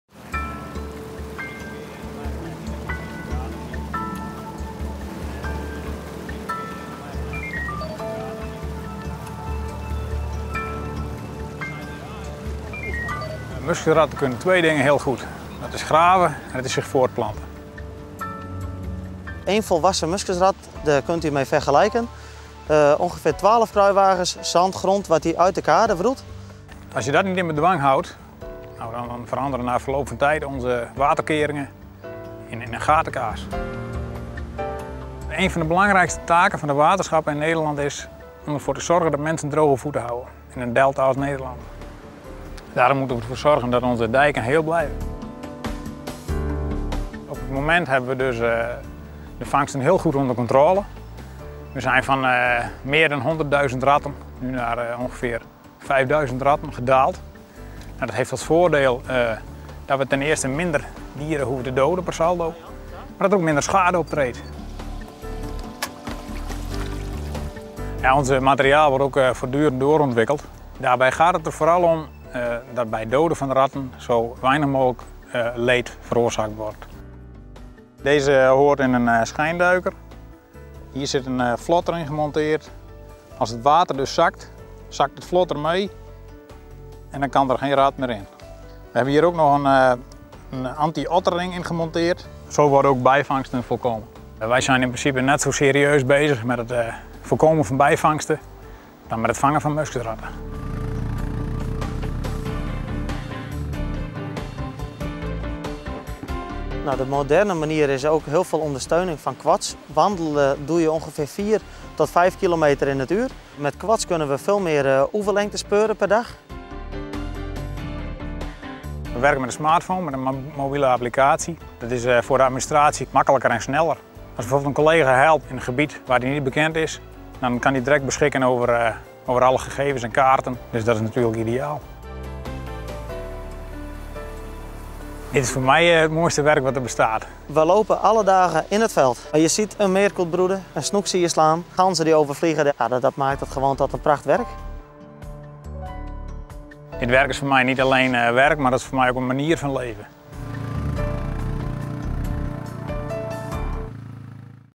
Muskusratten vangen
Muskusratten ondermijnen met hun gangen de dijken en kades. Simon Dijkstra en Jan van der Sluis zijn de hele dag op pad om deze gravers te vangen.